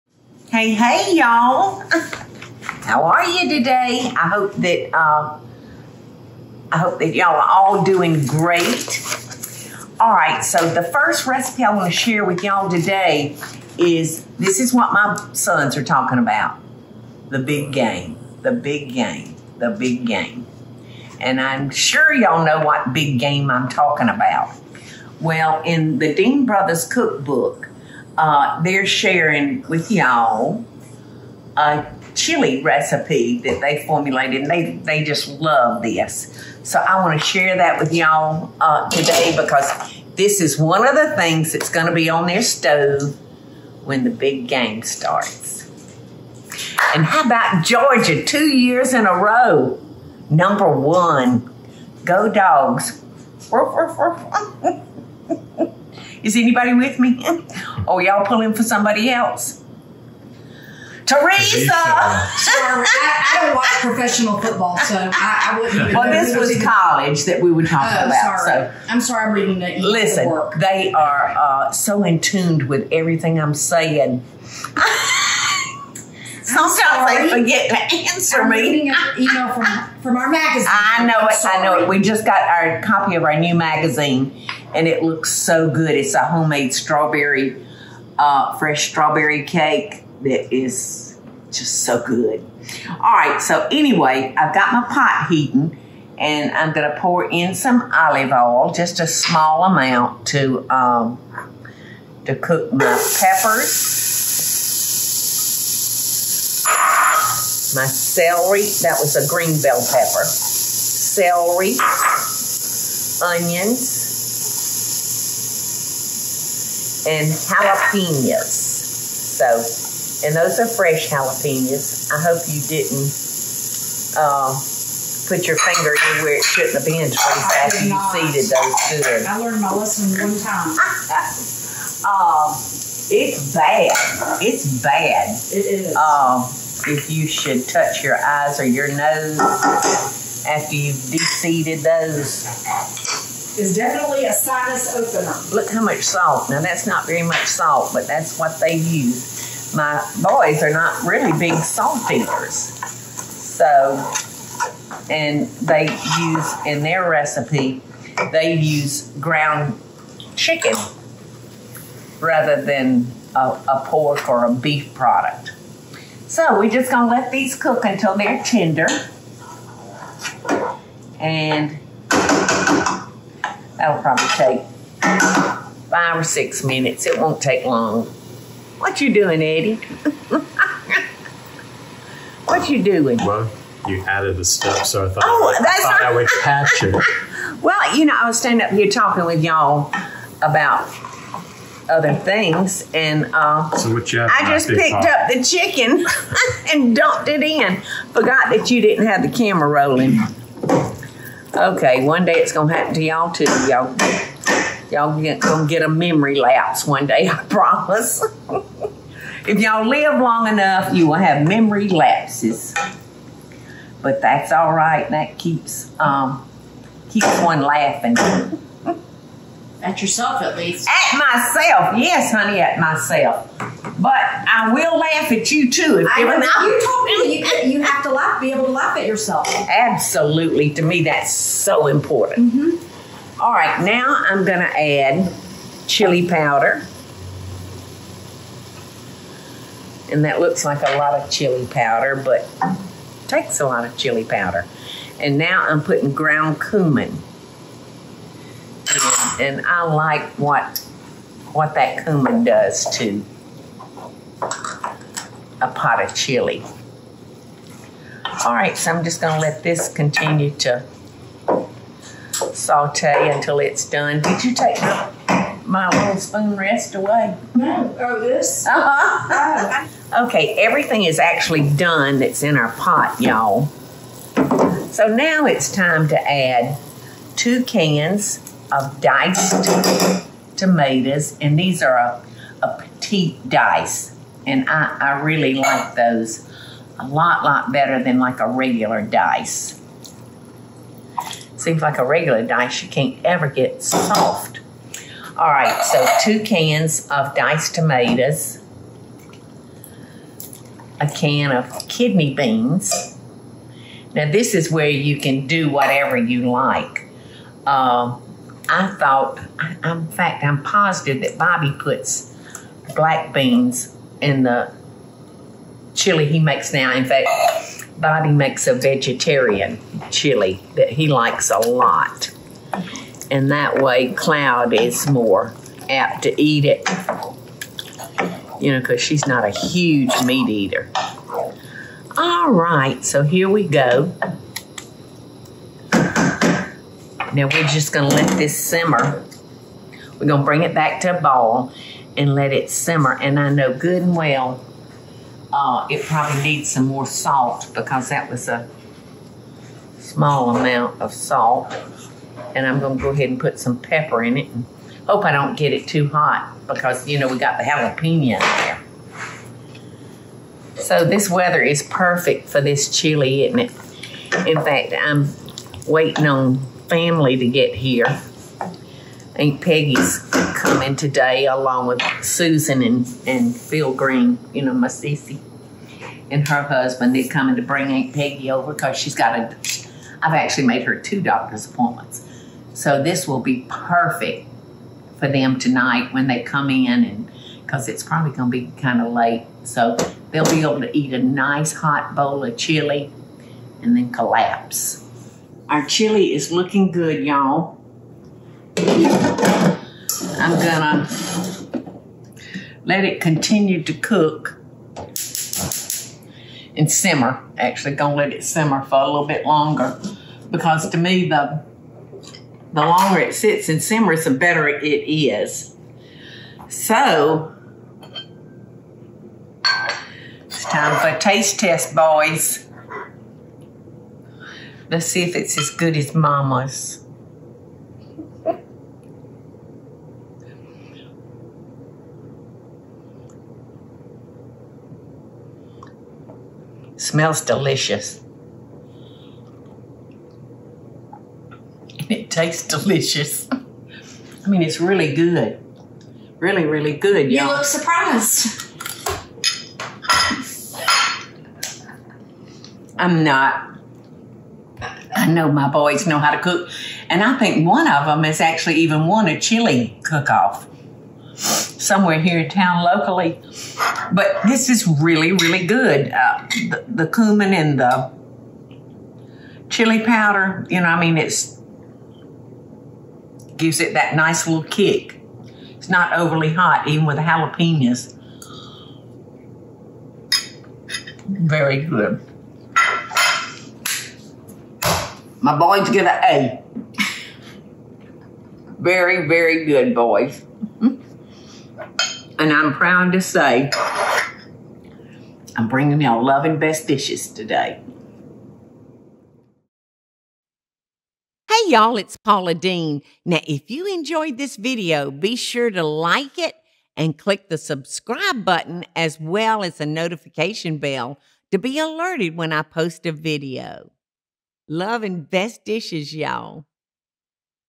Love & Best Dishes: Game Time Chili Recipe | Easy Ground Chicken Chili Recipe
Game Time Chili Recipe | Easy Ground Chicken Chili Recipe - Get easy recipes for dinner and great game day recipes from the Deen family, including this easy ground chicken chili recipe stove top!
Sign up for my email mailing list and get 3 FREE helpful kitchen printables